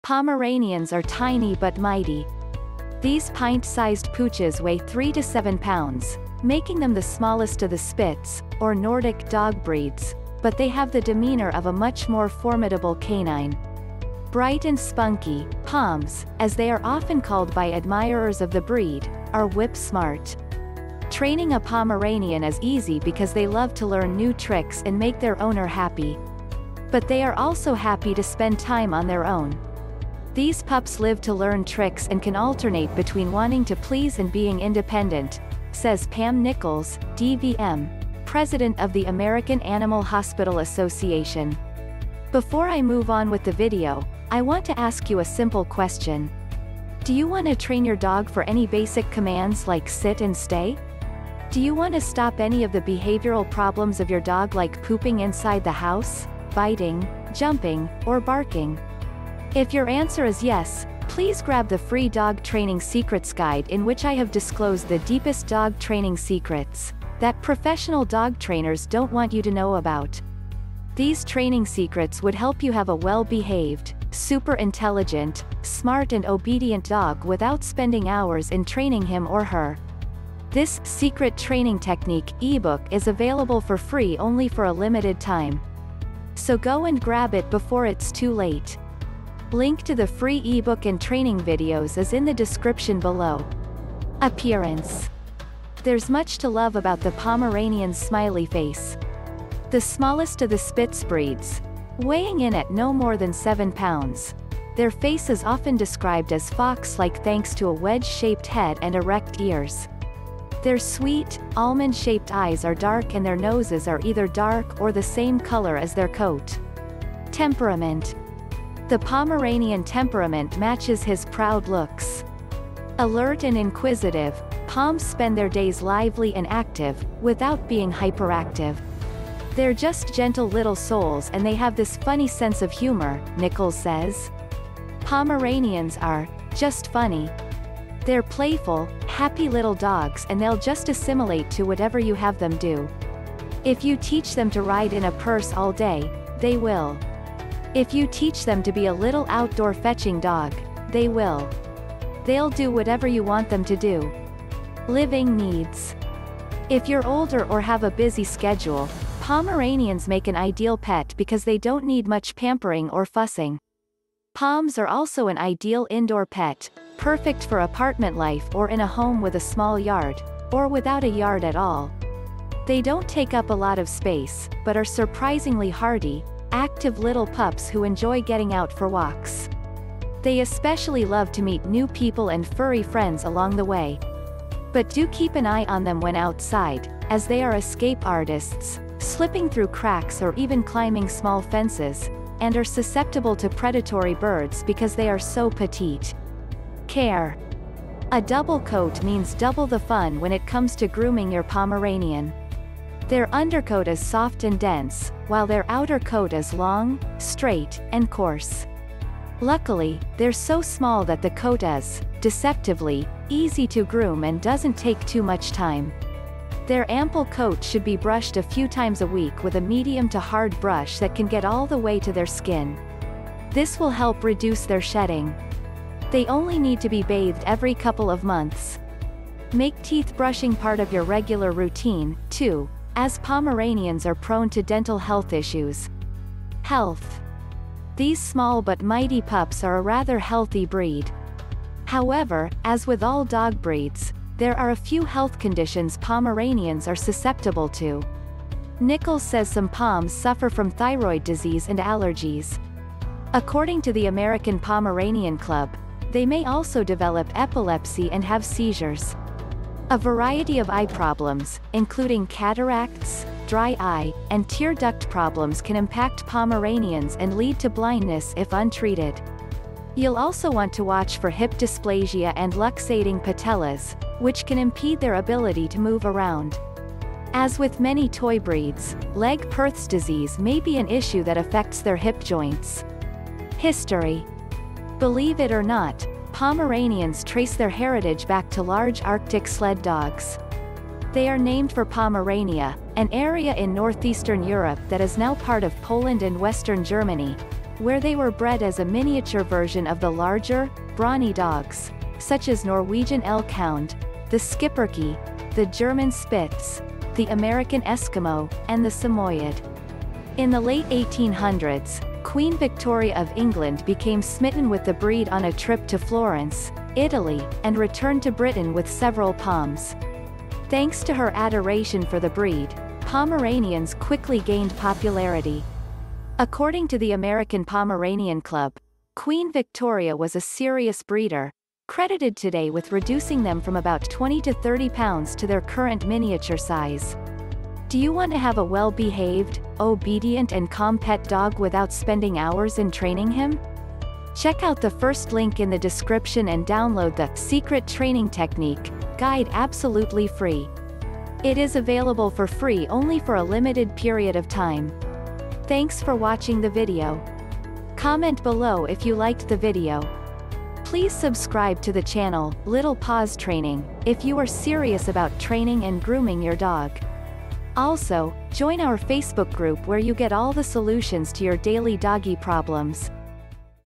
Pomeranian: Is this Dog Breed a Good choice for you and your Family?
Pomeranians are small in stature but big in personality. Alert, intelligent, and confident, Pomeranians have a beautiful coat and a charming personality to match. Read on to learn more about this little, loveable breed.

Pomeranians are tiny but mighty. These pint-sized pooches weigh 3–7 pounds, making them the smallest of the spitz (or Nordic) dog breeds, but they have the demeanor of a much more formidable canine. Bright and spunky, Poms, as they are often called by admirers of the breed, are whip-smart.

Training a Pomeranian is easy because they love to learn new tricks and make their owner happy. But they are also happy to spend time on their own. "These pups live to learn tricks and can alternate between wanting to please and being independent," says Pam Nichols, DVM, president of the American Animal Hospital Association.